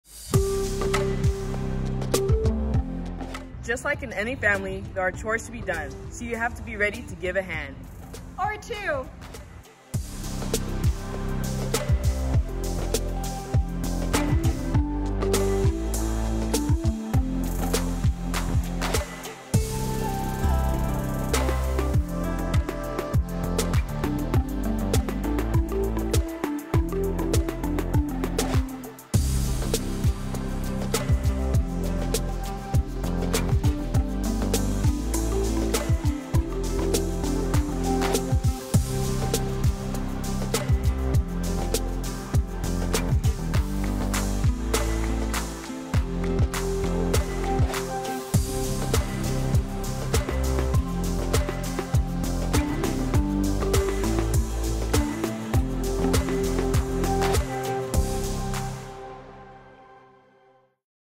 FIRST YEAR IN RELIGIOUS LIFE (Chores) - Salesian Sisters West Vocations
Just like in any family there are chores to be done so you have to be ready to give a hand... or two! Want to learn more about us?